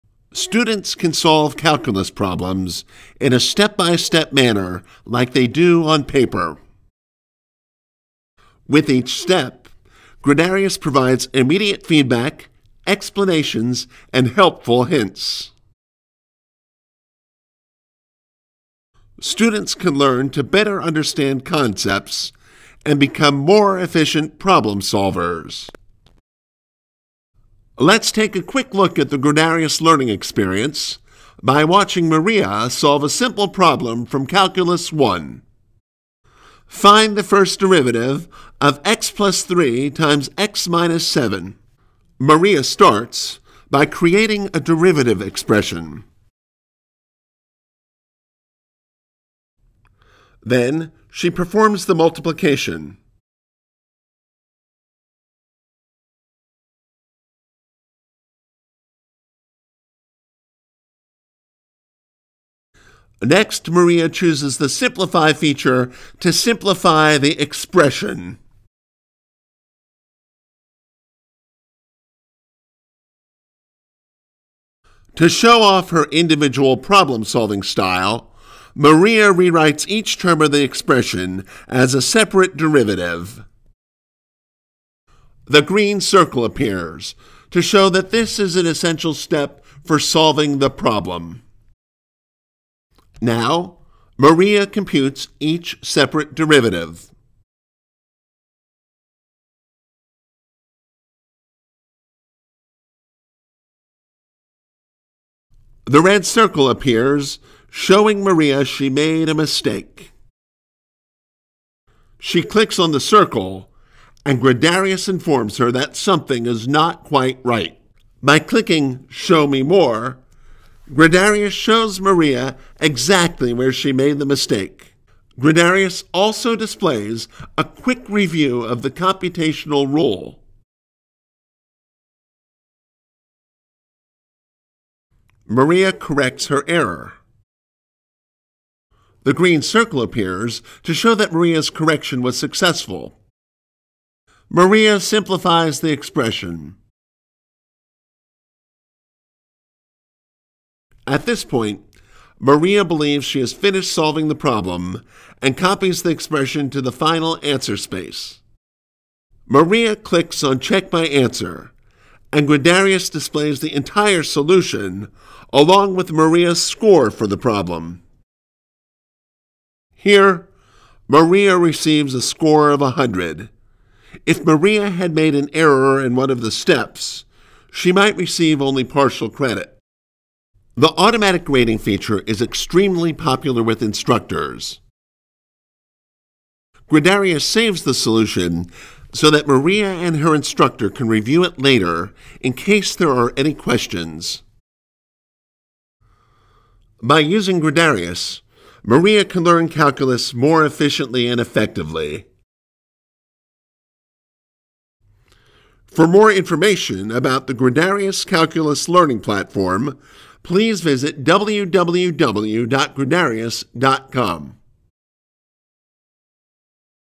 Calculus problem solving with Gradarius Learning Platform
An overview of the Gradarius learning experience.